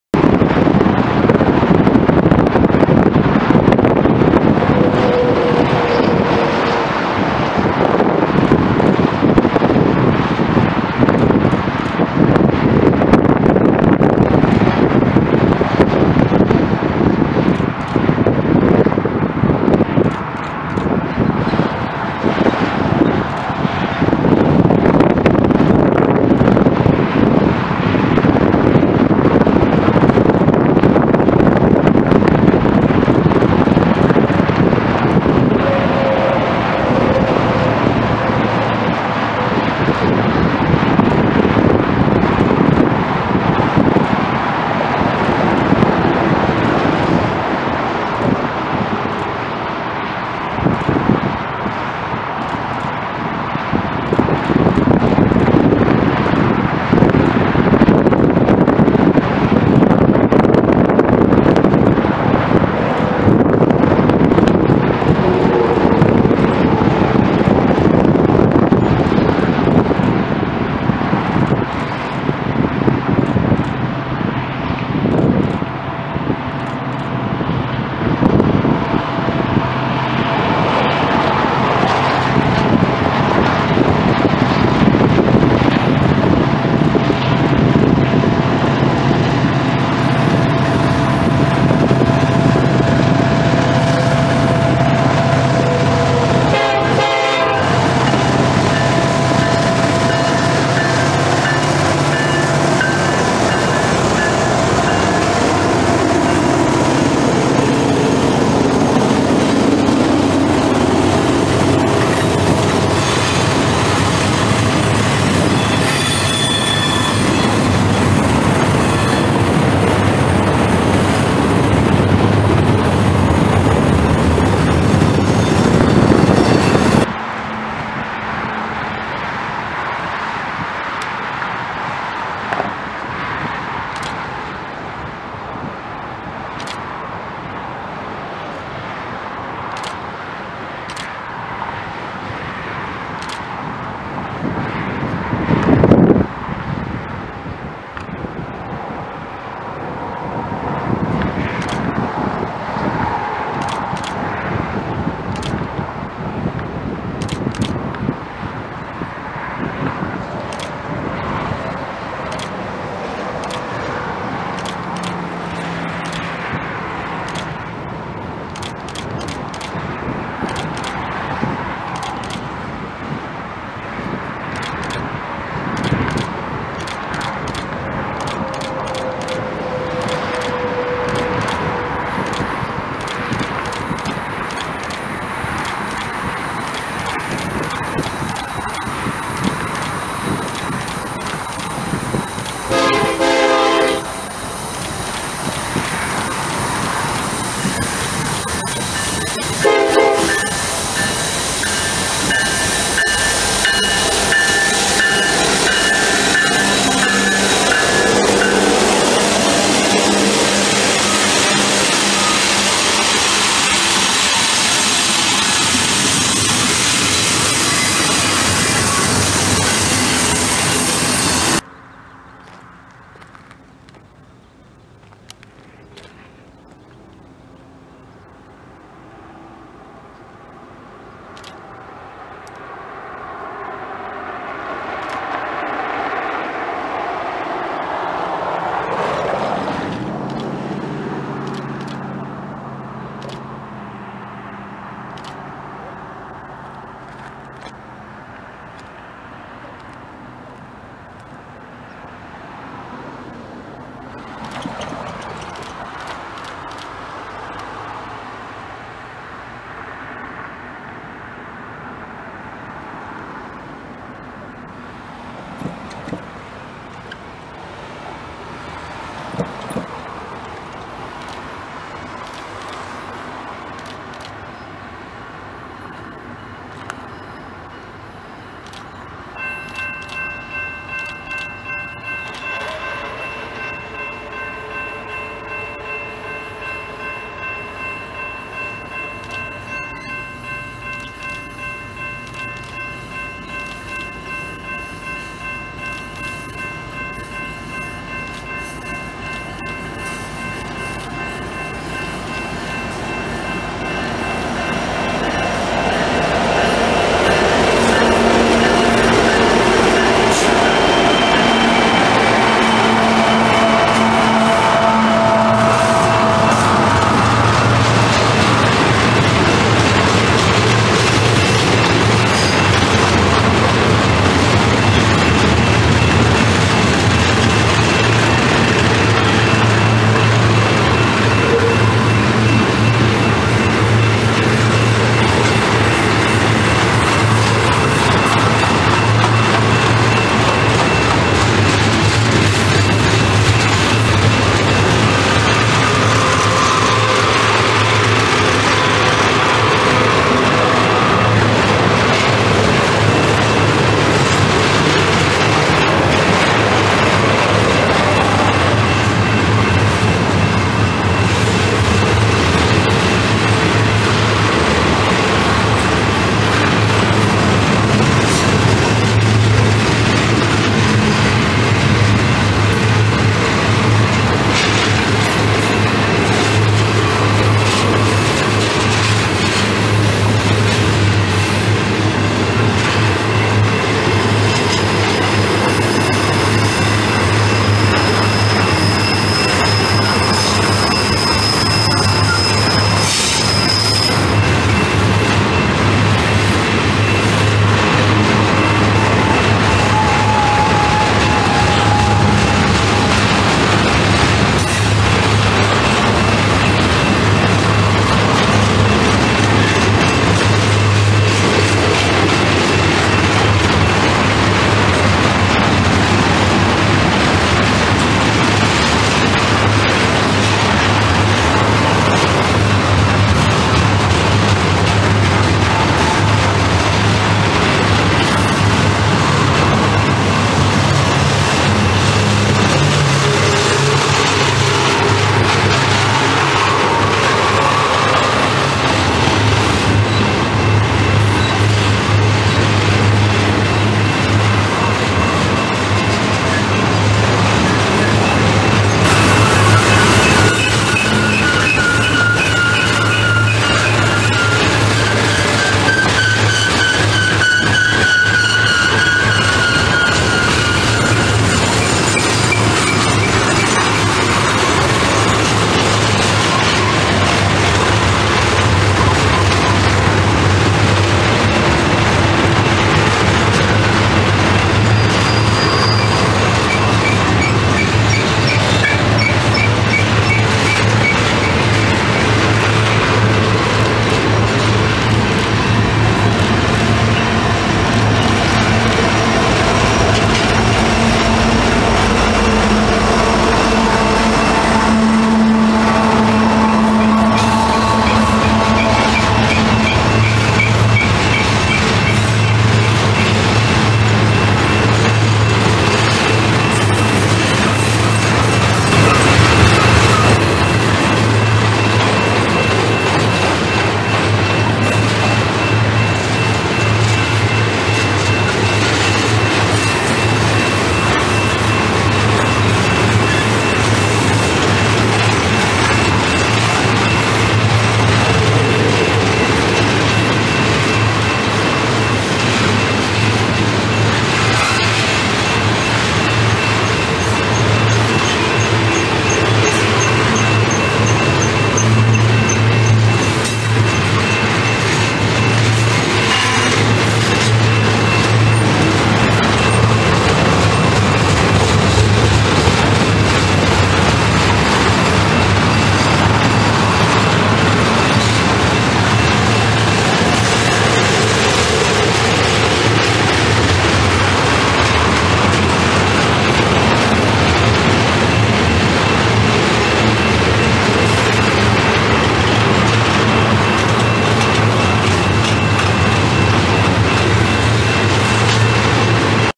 UP 1989 Threefer
Union Pacific 1989 (Denver and Rio Grande Western Heritage Unit) leads a Santa Teresa, NM to Long Beach, CA Intermodal Train on a Sunday afternoon and evening along the Yuma Subdivision.
First spot is at the long straight away near the old location known as Mons as the train grinds uphill in the Wind.
Second is at the top of San Timoteo Canyon (Big Curve at Beaumont) as the train starts up it's decent down the West Slope.
Third is more of a full train clip at the bottom of the Canyon just to the west of the old Redlands Crossovers.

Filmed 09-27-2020